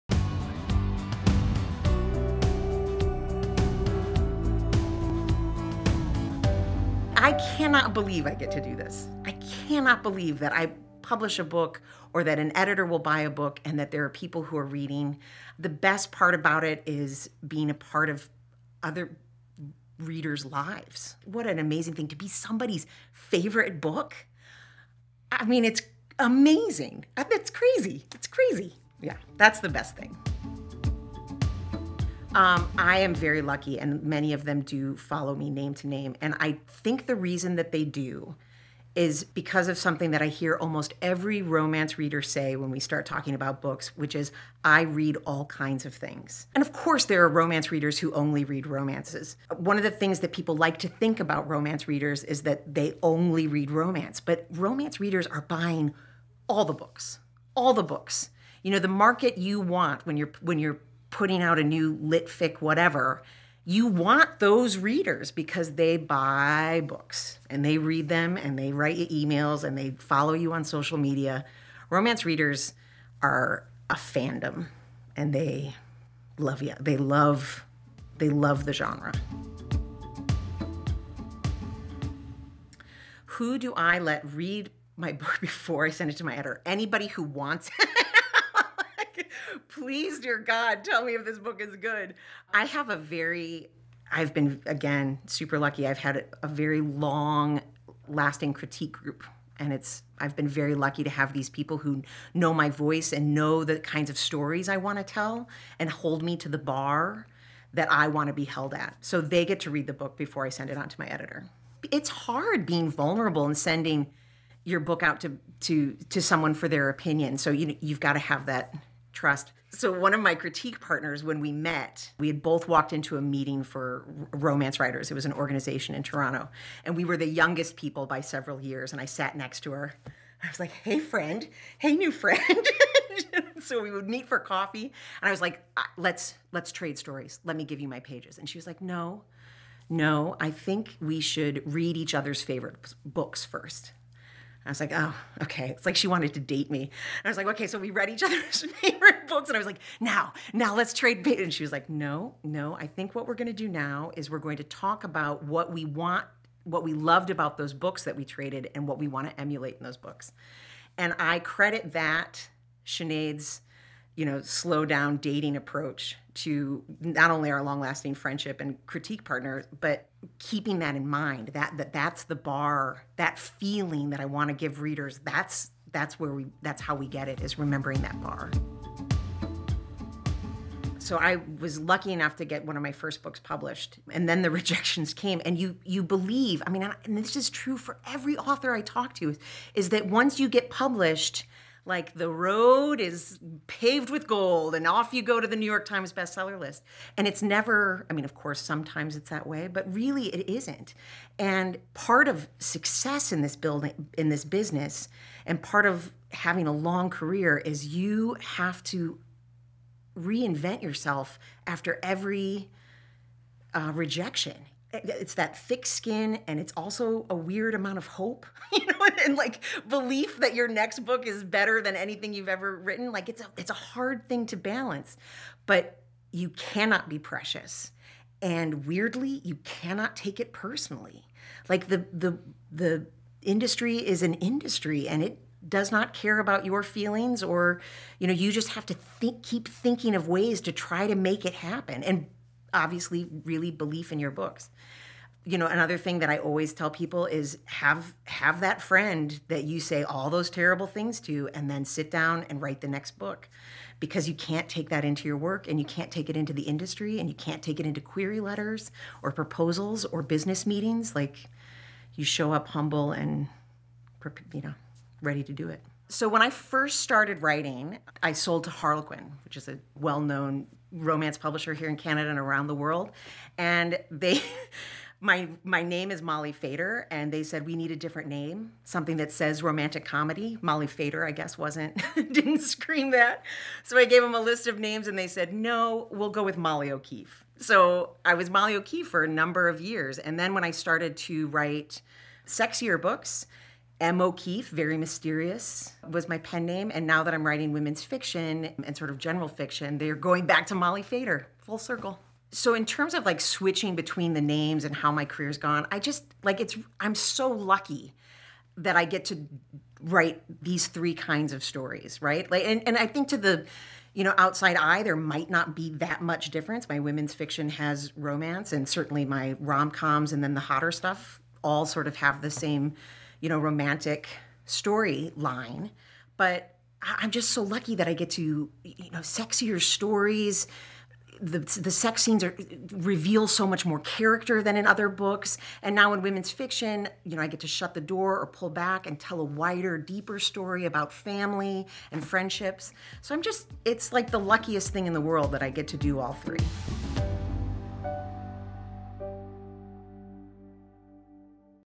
LUV | MEET THE AUTHOR: HOW TO OVERCOME PUBLISHING REJECTION WITH AUTHOR MOLLY FADER "O'KEEFE'
USA Today bestselling contemporary romance author, Molly Fader “O’Keefe,” tells us how she uses critique groups to improve her writing and her best advice in overcoming rejection from publishers.

Molly answers the following questions:

What’s the best part of being an author?
Tell us about the romance reading community.
Do you use critique groups?
How do you deal with rejection?
Why do you write under pen names?

Connect with Molly!

Join the FREE LUV app and CONNECT with the romance community today !